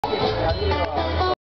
Taste of Greece 2011 -- Laila Aziz with Pericles
Laila Aziz performing With Pericles at the 2011 Taste of Greece in Chicago on Halsted on August 27, 2011